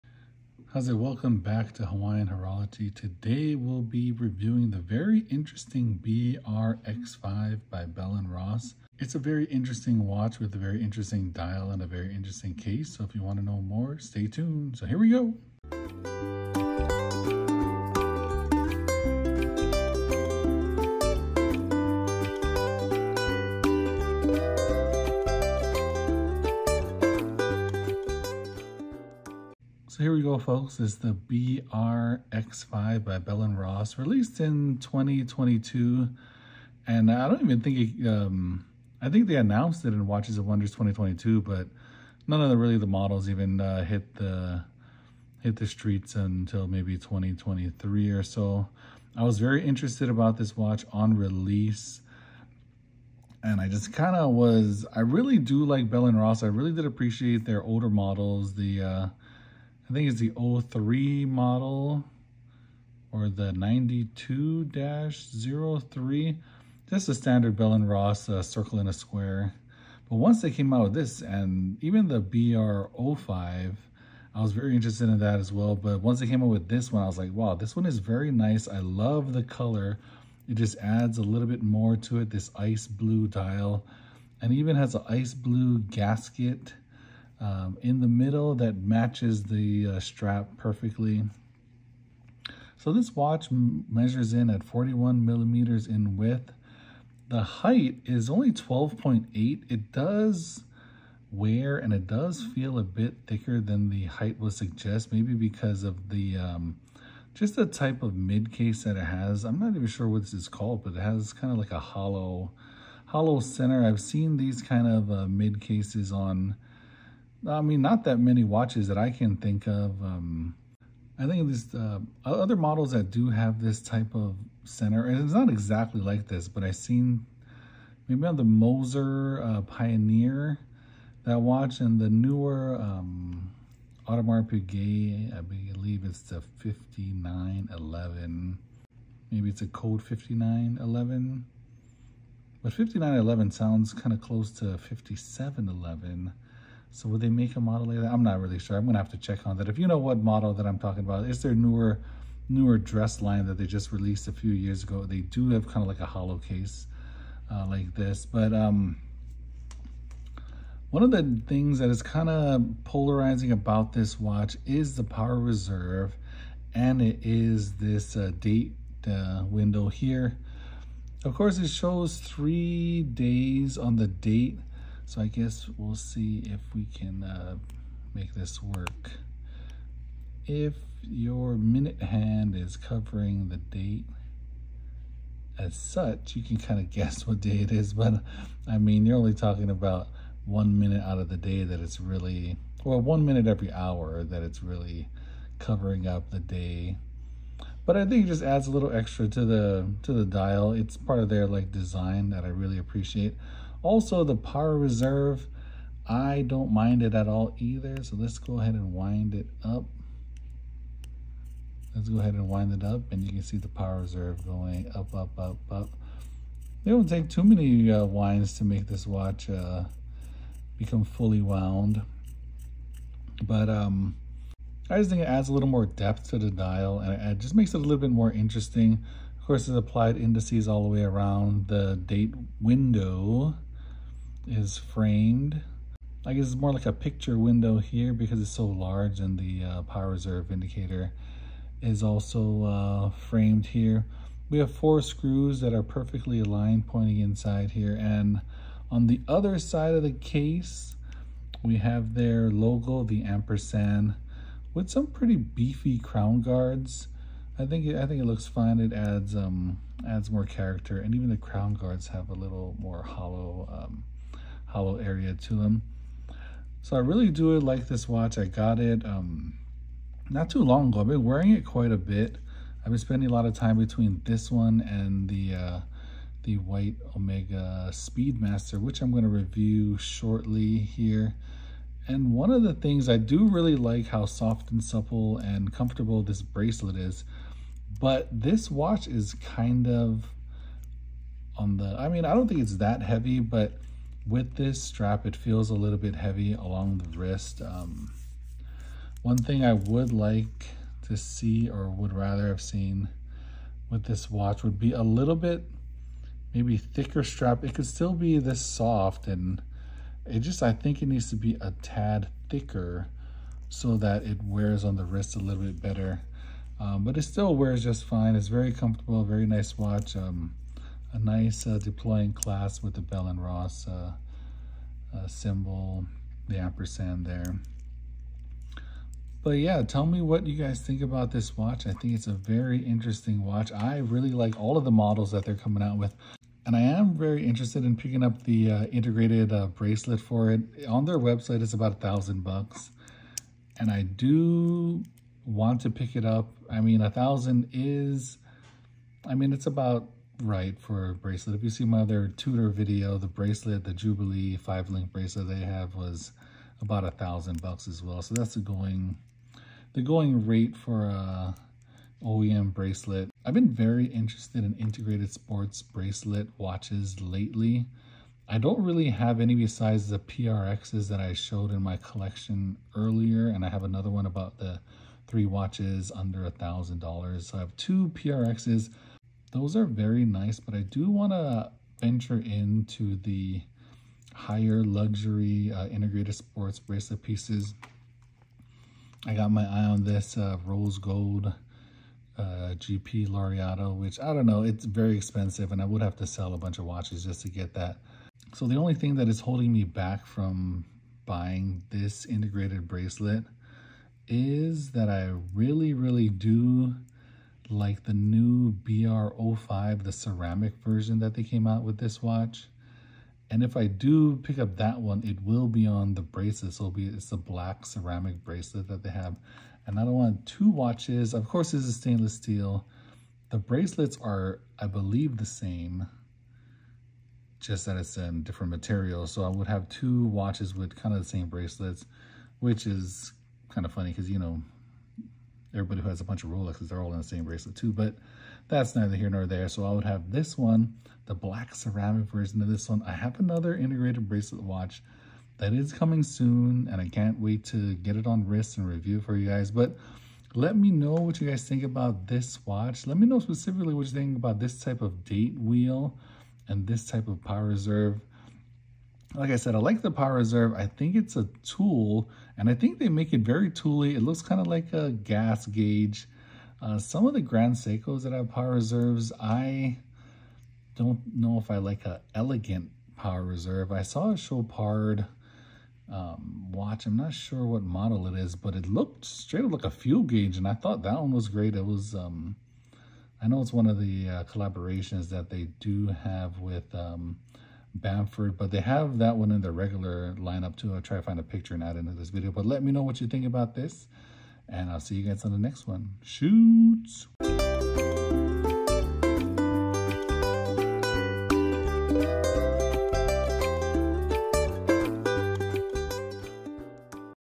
The most interesting square in the world! Bell & Ross BR-X5 Ice Blue Watch
Howzit today we have a special review of the BR-X5 by Bell & Ross in ice blue their signature circle in a square with some amazing upgrades!  This watch caught my eye when it was first released and it did not disappoint one bit in the metal.  If you get the chance check this one out.  And sorry for not releasing regular updates but you can expect more videos moving forward!